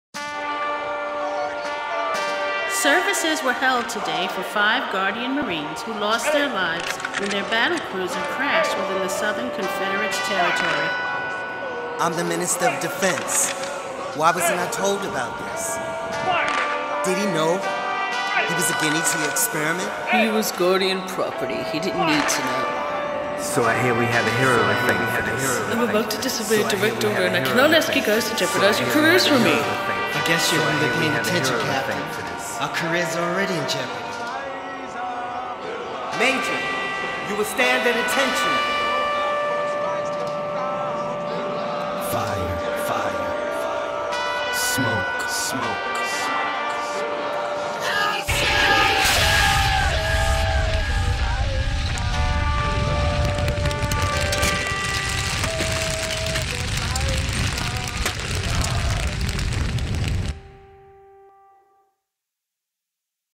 ASHES Book Trailer
ASHES is a sci-fi book written by Jackson Terrow.